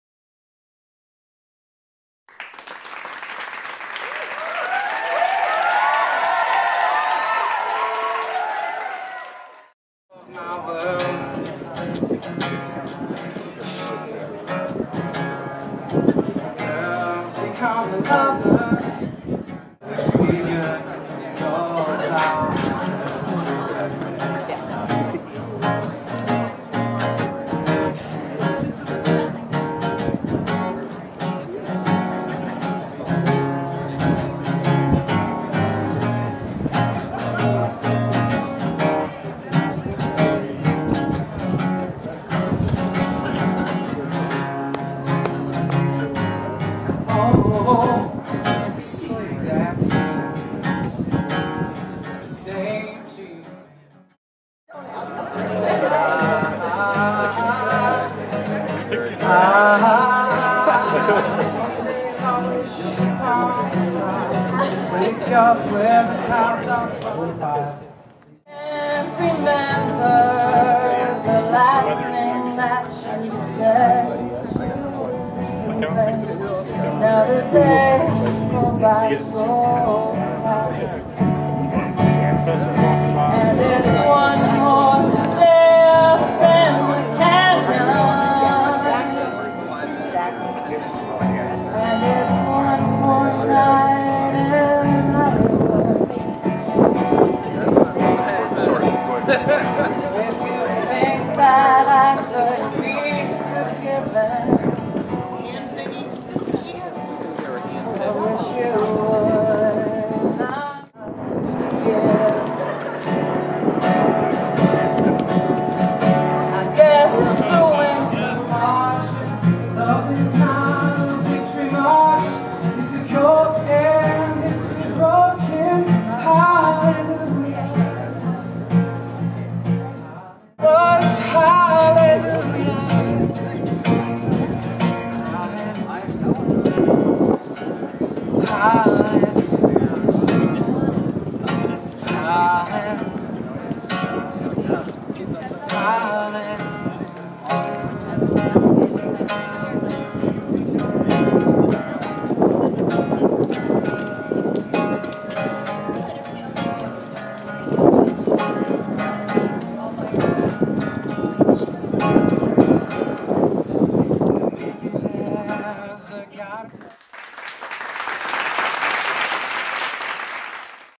Campus Coffee House Series
Campus Coffee House Series - An acoustic blend of talent & taste each Wednesday at Ohio State Marion.

FREE coffee & acoustic entertainment for Marion campus students, faculty, staff, and the community at large.